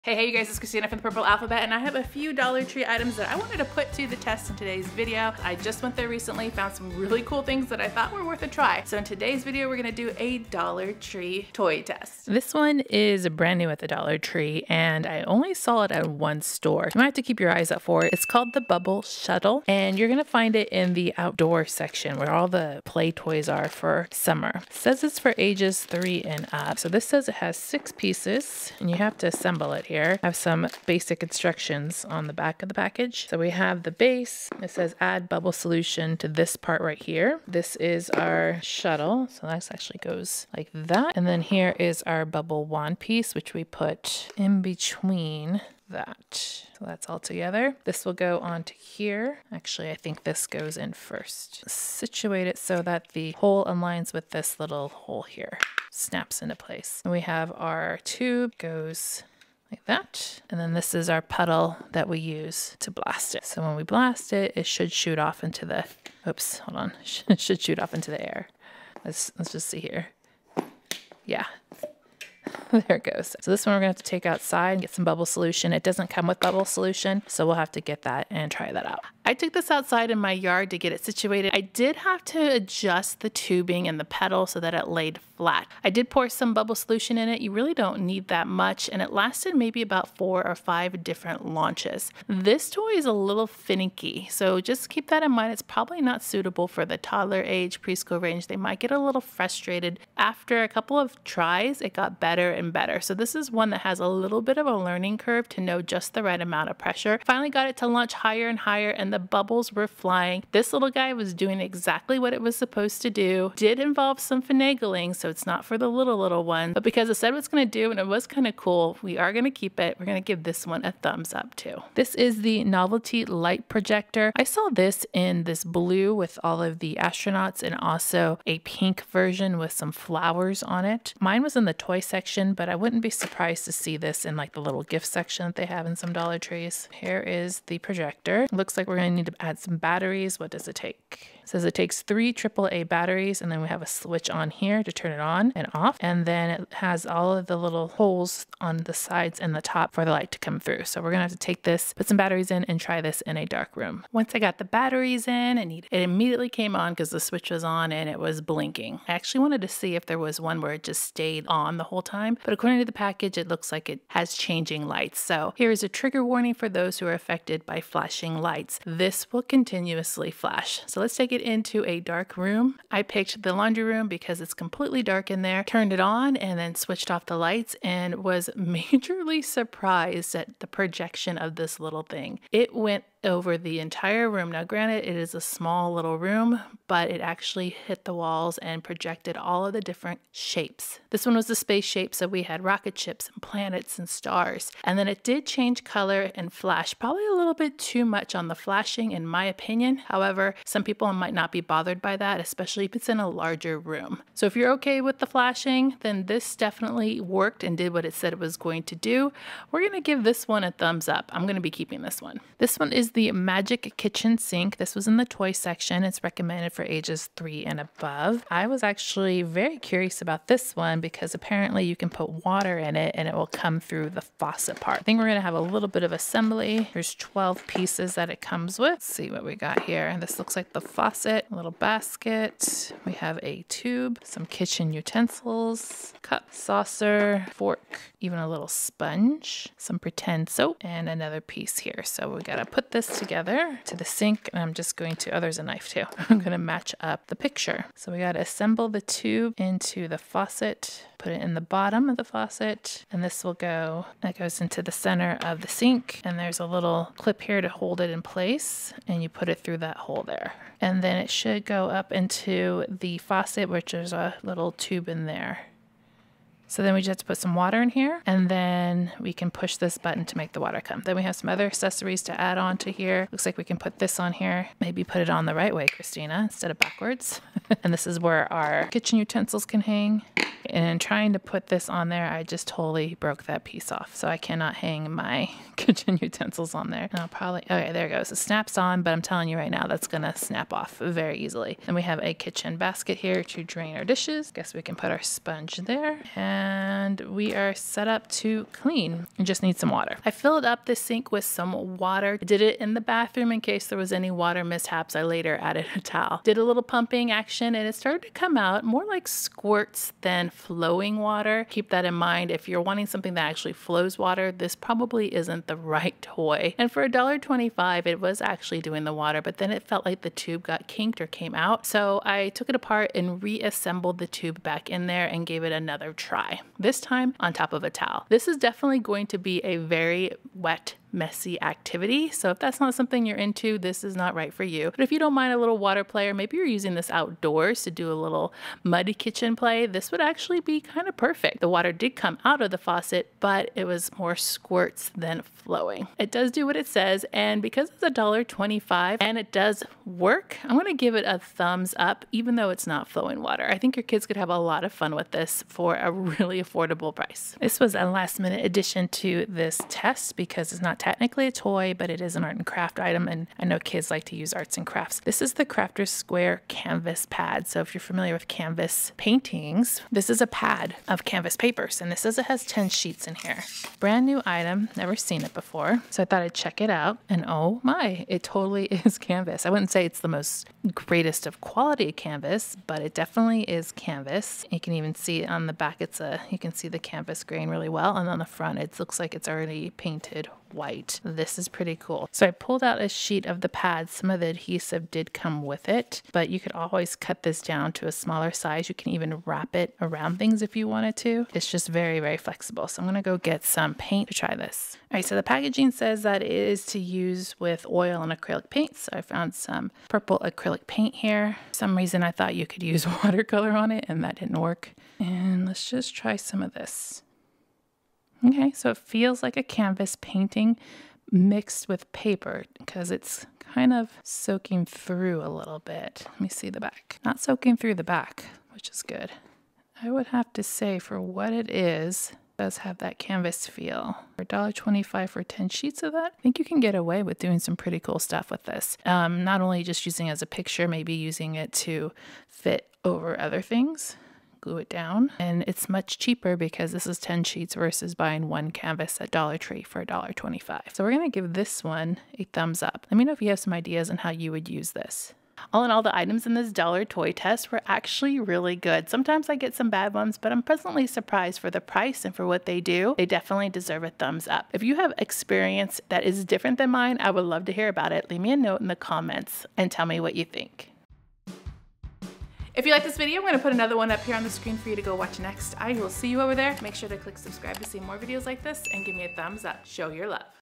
I Put DOLLAR TREE to the Test and Found the GOOD ONES
I picked out some items at Dollar Tree, took them home and then put them to the test. I found some really fun things for kids that REALLY work. This video is filled with some great finds at Dollar Tree that are worth the buy.